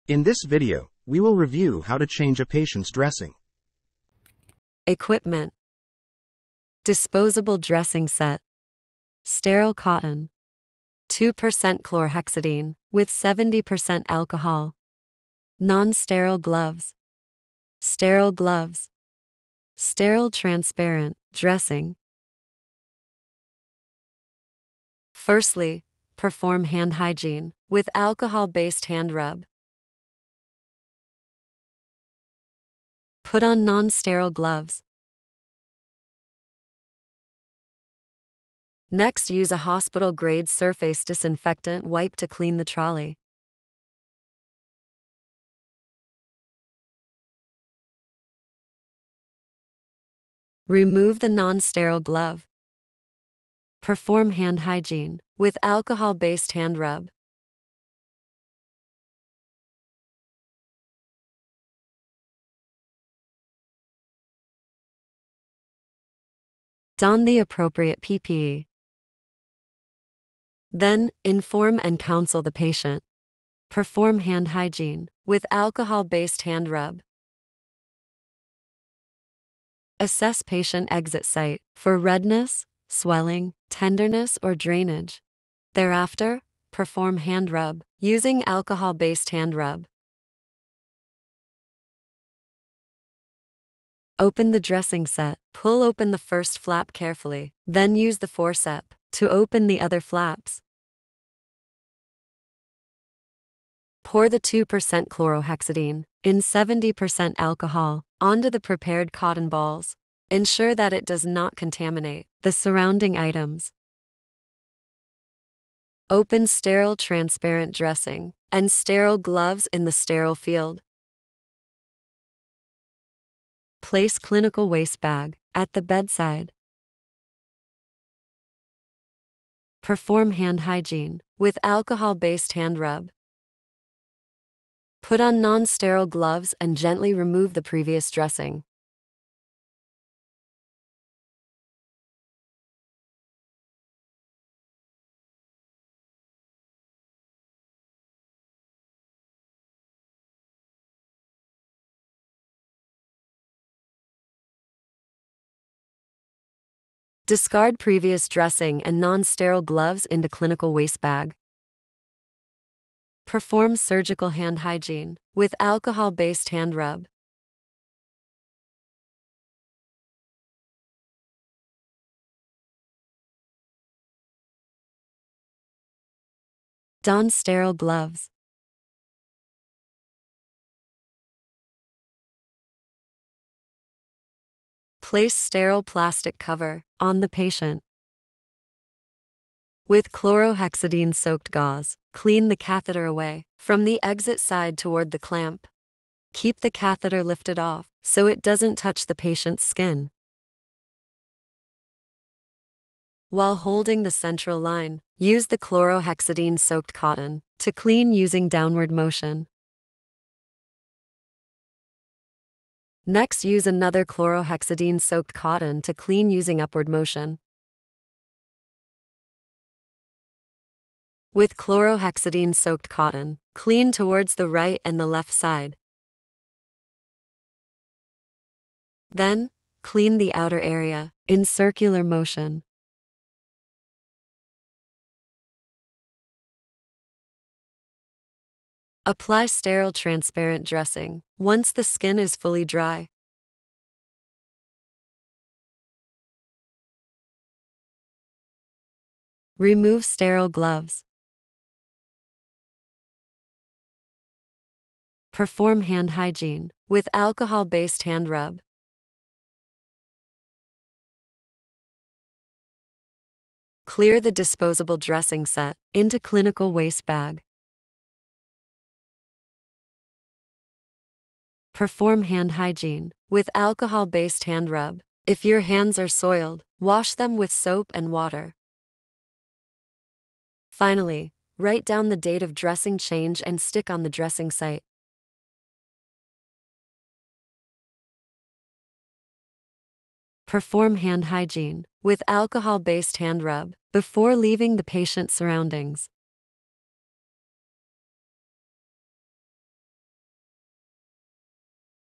Central Venous Catheter (CVC) Dressing Change
CVC plays a crucial role in modern healthcare, facilitating various medical procedures and treatments. However, improper maintenance and care can lead to complications such as central line-associated bloodstream infection (CLABSI). As a healthcare worker it is important to ensure the safety and well-being of the patient with a catheter. In this video, you will be able to understand and implement the correct techniques for changing dressings on central venous catheter (CVC), ensuring adherence to infection control protocols and patient safety standards.

The central venous catheter (CVC) dressing change video is prepared by the infection prevention and control (IPC) team of University Malaya Medical Centre (UMMC) in collaboration with Hospital Kuala Lumpur (HKL), Hospital Tengku Ampuan Rahimah (HTAR), and Hospital Queen Elizabeth II (HQEII).

This video is an initiative of the Malaysia Evaluation and Recommendation for Infection Control (MY ERIC) project, funded under the Transdisciplinary Research Grant Scheme (TRGS/1/2020/UM/02/2).

The dressing change video was filmed and reviewed in April 2023.
Remember to always perform Hand Hygiene, Scrub the Hub for 30 twist or 15 seconds and Change dressing every 7 days for transparent semi-preamble, every 2 days for gauze or whenever the site appears loose, soiled, or wet.

For further details regarding the terms and definition of CLABSI and the various types of central lines, please refer to the Centers for Disease Control and Prevention (CDC) manual, available